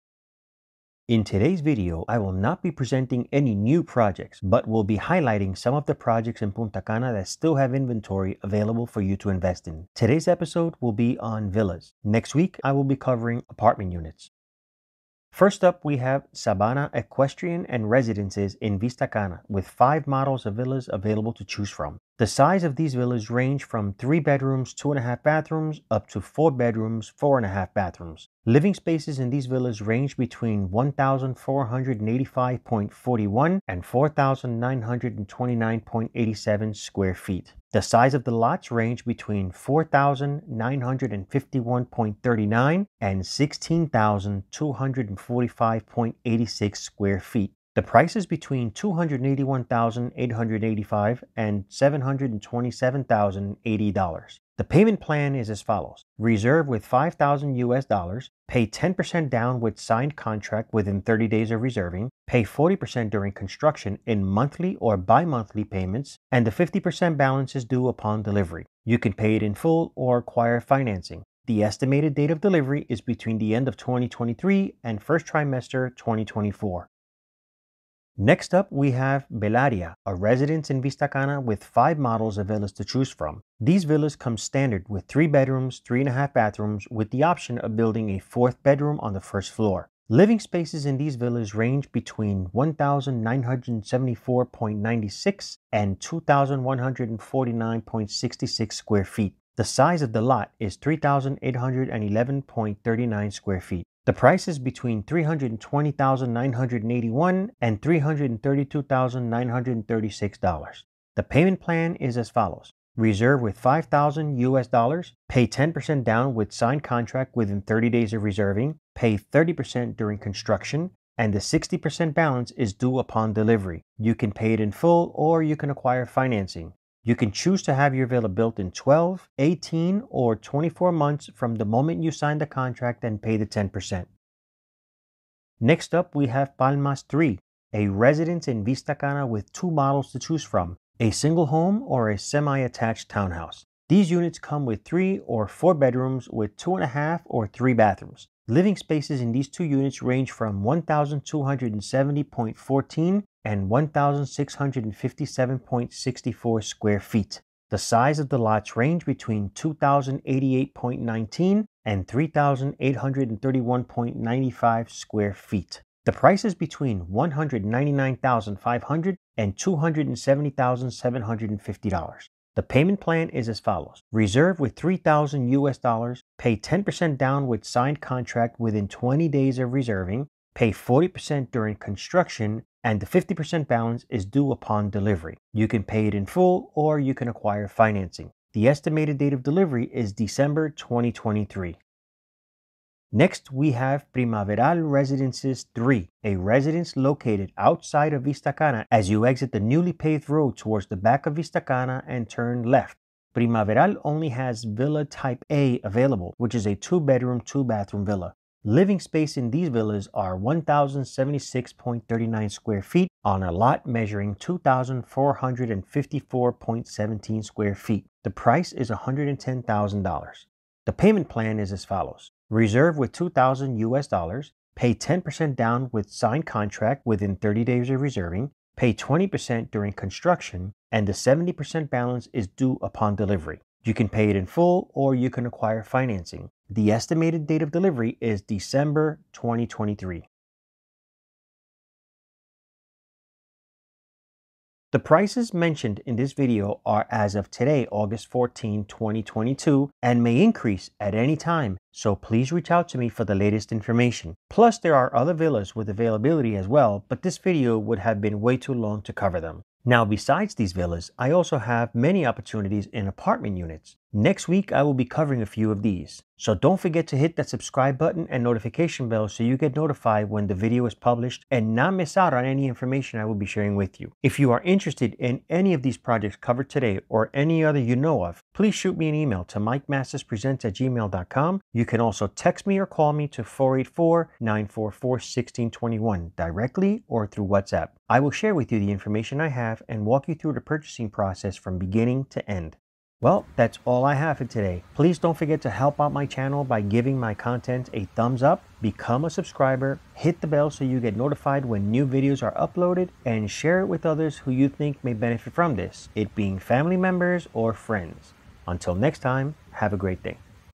Villas In Punta Cana. What's Available?
This video only touched up on 4 different projects of villas, 3 of them being in Vista Cana, and 1 of them right behind Vista Cana. There are still opportunities present for those in a position ready to purchase. Prices are still competitive, even though prices have gone up since the Covid-19 Pandemic. I have a list of apartments for sale as well in various projects ranging from studios, 1 bedroom, 2 bedrooms and 3 bedrooms.